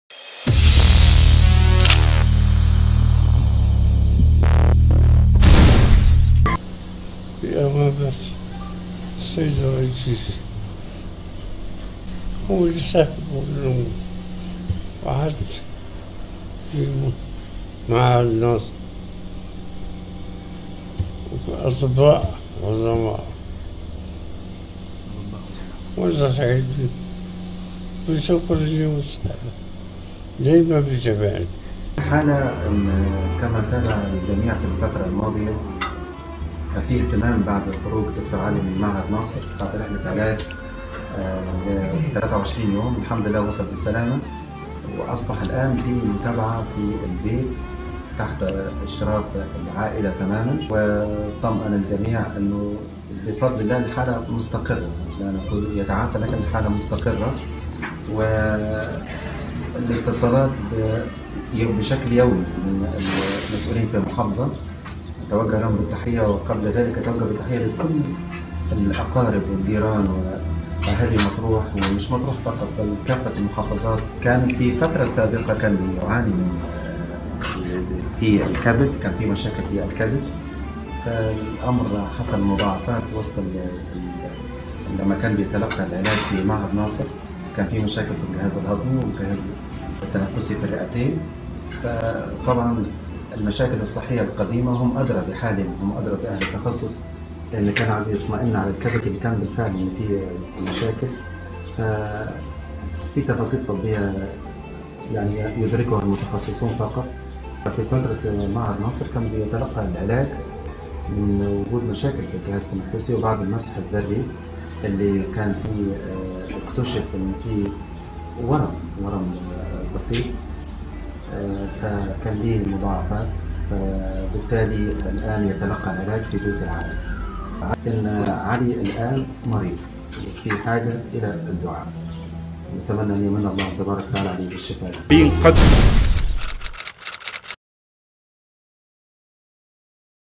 شاهد اللحظات الأخيرة للفنان علي حميدة واَخر كلماته قبل رحيله اليوم.. وماذا قال شقيقه عن حالته
جوو_2_فن / شاهد اللحظات الأخيرة للفنان علي حميدة واَخر كلماته قبل رحيله اليوم.. وماذا قال شقيقه عن حالته